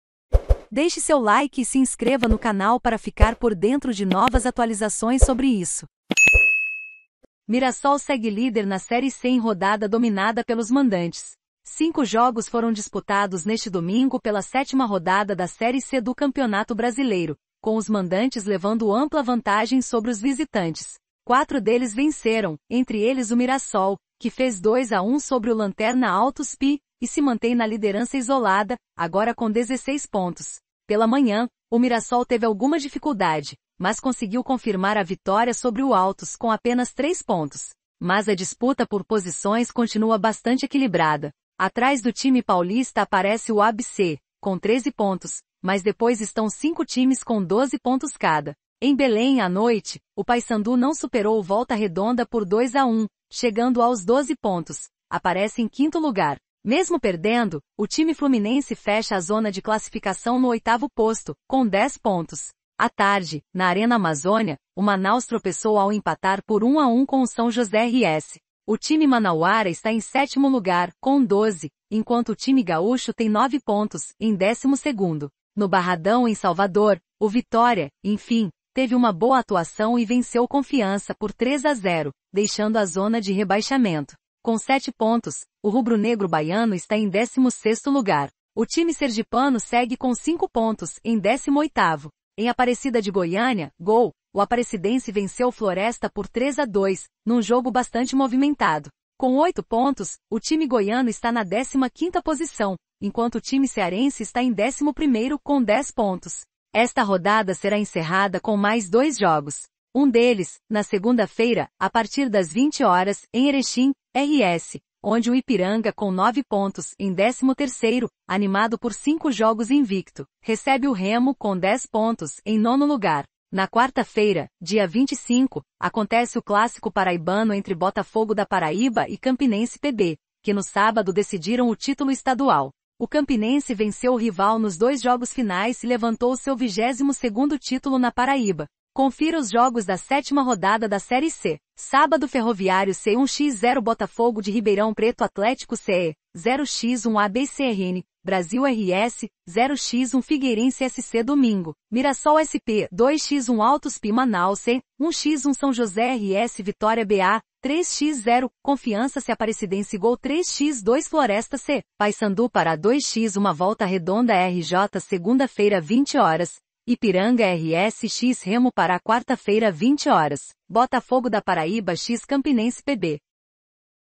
MIRASSOL SEGUE LÍDER NA SÉRIE C EM RODADA DOMINADA PELOS MANDANTES
Cinco jogos foram disputados neste domingo pela sétima rodada da Série C do Campeonato Brasileiro. com os mandantes levando ampla vantagem sobre os visitantes. Quatro deles venceram. entre eles o Mirassol. que fez 2 a 1 sobre o lanterna Altos PI e se mantém na liderança isolada. agora com 16 pontos. Pela manhã. o Mirassol teve alguma dificuldade. mas conseguiu confirmar a vitória sobre o Altos. com apenas três pontos. Mas a disputa por posições continua bastante equilibrada. Atrás do time paulista aparece o ABC. com 13 pontos. mas depois estão cinco times com 12 pontos cada. Em Belém. à noite. o Paysandu não superou o Volta Redonda por 2 a 1. chegando aos 12 pontos   aparece em quinto lugar. Mesmo perdendo. o time fluminense fecha a zona de classificação no oitavo posto. com 10 pontos. À tarde. na Arena Amazônia. o Manaus tropeçou ao empatar por 1 a 1 com o São José RS. O time manauara está em sétimo lugar. com 12. enquanto o time gaúcho tem nove pontos. em 12º. No Barradão. em Salvador. o Vitória. enfim. teve uma boa atuação e venceu o Confiança. por 3 a 0. deixando a zona de rebaixamento. Com sete pontos. o rubro negro baiano está em 16º lugar. O time sergipano segue com cinco pontos. em 18º. Em Aparecida de Goiânia (GO). o Aparecidense venceu o Floresta por 3 a 2. num jogo bastante movimentado. Com oito pontos. o time goiano está na 15ª posição. enquanto o time cearense está em 11º. com 10 pontos. Esta rodada será encerrada com mais dois jogos. Um deles. na segunda feira. a partir das 20h. em Erechim (RS). onde o Ypiranga. com nove pontos. em 13º. animado por cinco jogos invicto. recebe o Remo. com 10 pontos. em nono lugar. Na quarta feira. dia 25. acontece o clássico paraibano entre Botafogo PB e Campinense PB. que no sábado decidiram o título estadual. O Campinense venceu o rival nos dois jogos finais e levantou o seu 22º título na Paraíba. Confira os jogos da 7ª rodada da Série C: SÁBADO Ferroviário CE 1 x 0 Botafogo SP Atlético CE 0 x 1 ABC RN Brasil RS 0 x 1 Figueirense SC DOMINGO Mirassol SP 2 x 1 Altos PI Manaus AM 1 x 1 São José RS Vitória BA 3 x 0 Confiança SE Aparecidense GO 3 x 2 Floresta CE Paysandu PA 2 x 1 Volta Redonda RJ SEGUNDA FEIRA 20h   Ypiranga RS x Remo PA QUARTA FEIRA 20h   Botafogo PB x Campinense PB